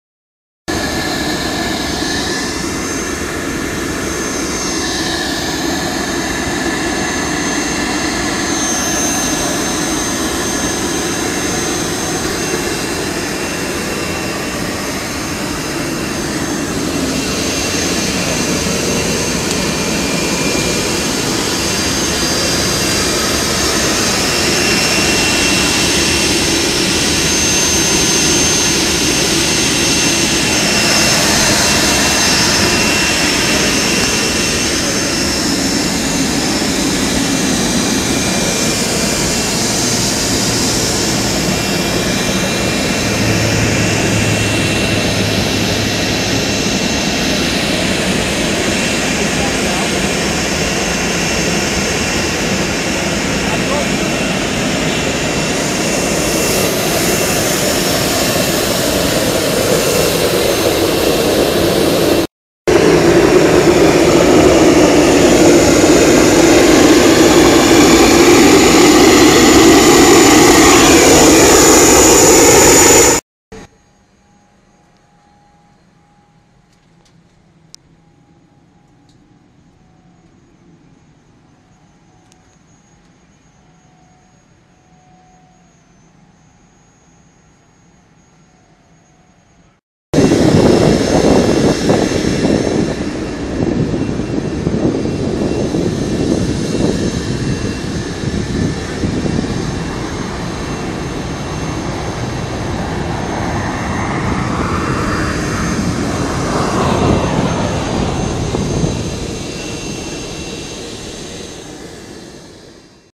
Walkaround and inside Swift Air N802TJ 737-400 at Phoenix Sky Harbor KPHX
737-4B7 operated by Swift Airlines. Ex US Airways at Swift Ramp